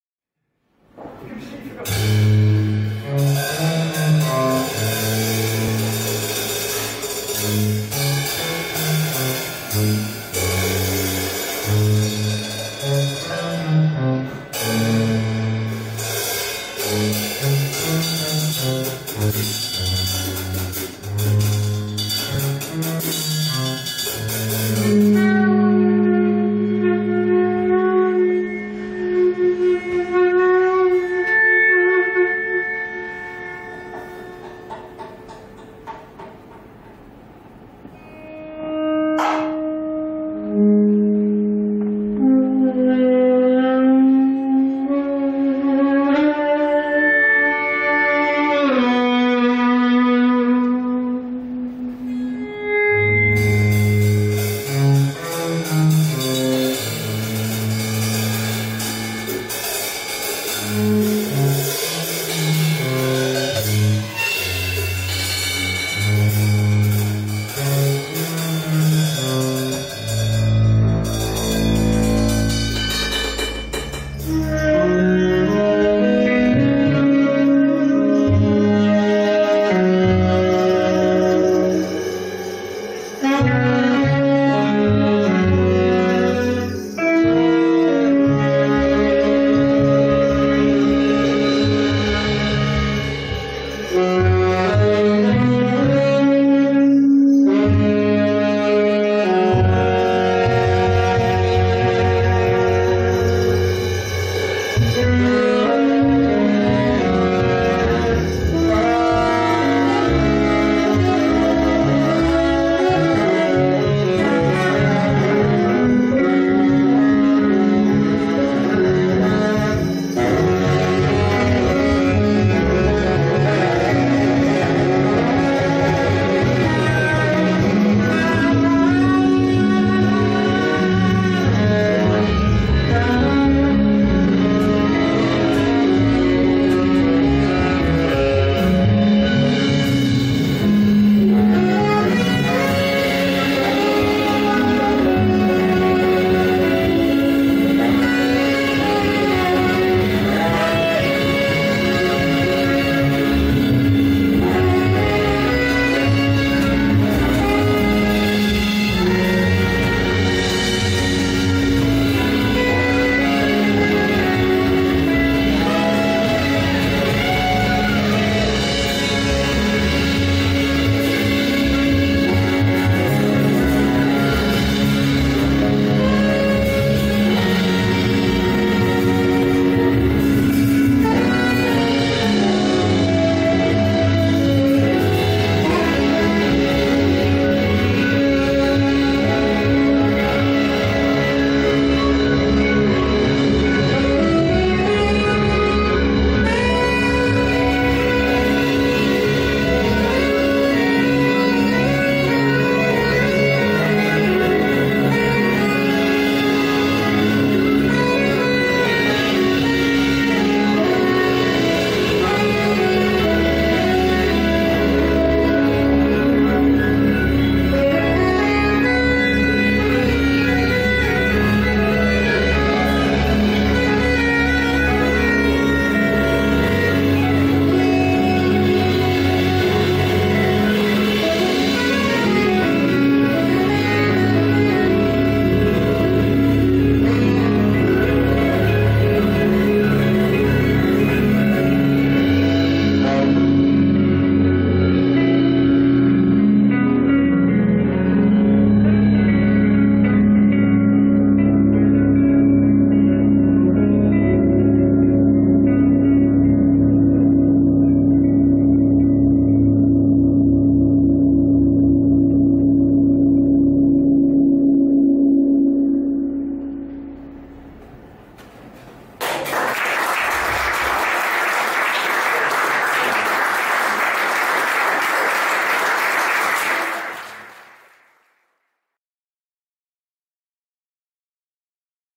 Sloth Racket #1 (excerpt)
Last piece - end of Set #2

Recorded live at The Bridge Hotel, Newcastle  September 2016

  Cath Roberts (baritone sax)
  Sam Andreae (tenor sax)
  Seth Bennett (bass)
  Johnny Hunter (drums)
  Anton Hunter (guitar)

  Cameras & video editing by Ken Drew

A Jazz North East presentation